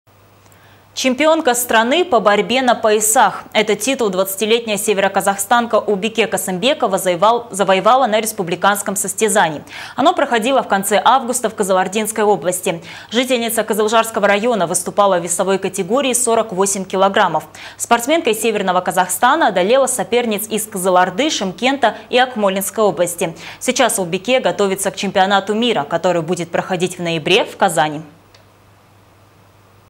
Чемпионом страны по борьбе на поясах стала У.Касымбекова из СКО
Чемпионка страны по борьбе на поясах. Этот титул 20-летняя североказахстанка Улбике Касымбекова завоевала на республиканском состязании. Оно проходило в конце августа в Кызылординской области.